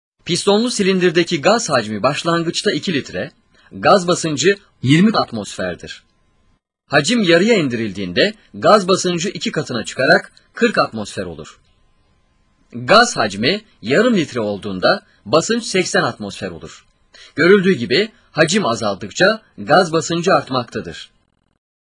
KAPALI KAPLARDAKİ GAZLARIN BASINÇ HACİM İLİŞKİSİ-1.avi
Murat Üstündağ-Kayseri Mithatpaşa İlköğretim Okulu Fen ve Teknoloji Öğretmeni-KAPALI KAPLARDAKİ GAZLARIN BASINÇ HACİM İLİŞKİSİ-1